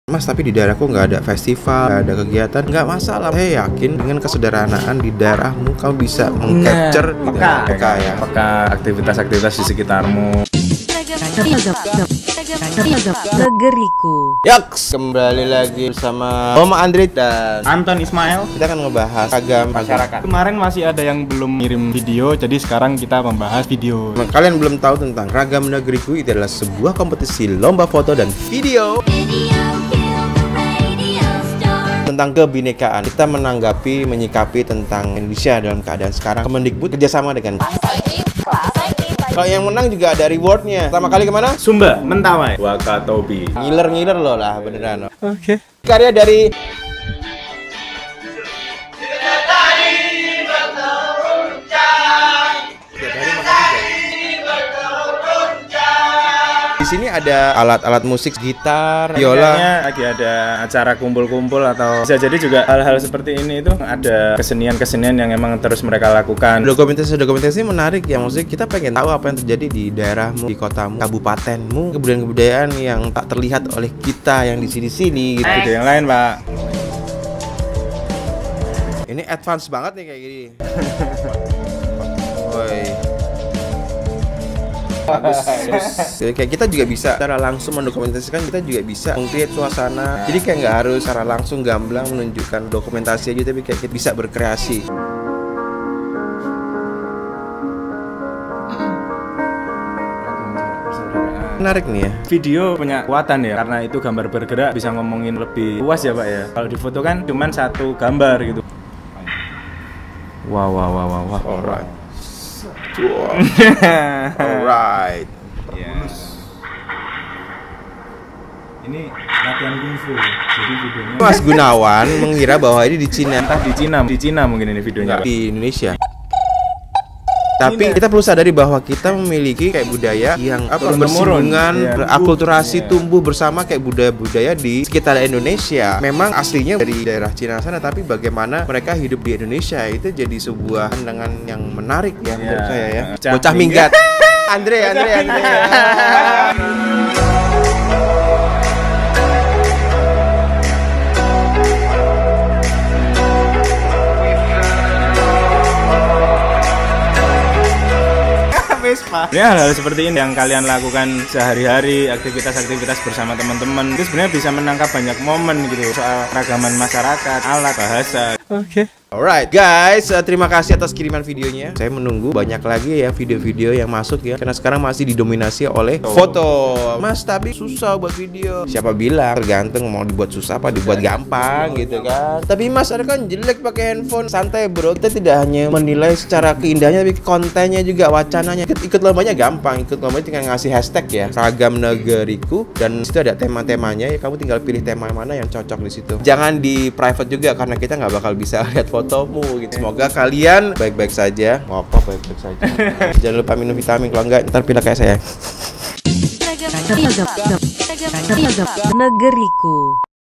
NGOMENTARIN FOTO-VIDEO RAGAMNEGERIKU (#02)
Kembali lagi dengan video review ragamnegeriku ke-2. Inilah beberapa submission yang sudah masuk ke Instagram @ragamnegeriku dan @kelaspagijkt yang direview langsung oleh Pa'e Anton Ismael dan tim Kelas Pagi dengan tema "kekerabatan dan organisasi kemasyarakatan"! Gimana? Ada foto atau video kamu di sana?

Buat kamu yang belum submit foto atau video, masih ada waktu buat submit foto atau video bertema "kekerabatan dan organisasi kemasyarakatan". Ingat ya, nggak harus kekerabatan dan organisasi kesenian dan adat, bisa juga tentang moment-moment menarik yang ada di sekitar kamu. Dan juga ingat pesan Pa'e dan tim Kelas Pagi, tambahkan caption yang menarik dan lebih komunikatif! Posting dengan hashtag ragamnegeriku dan ragammasyarakatindonesia lalu tag IG @ragamnegeriku. Jangan lupa juga follow IG @ragamnegeriku dan @kelaspagijkt ya!

Semoga kamu yang beruntung untuk menikmati paket perjalanan wisata ke Sumba dan 2 tempat wisata lainnya!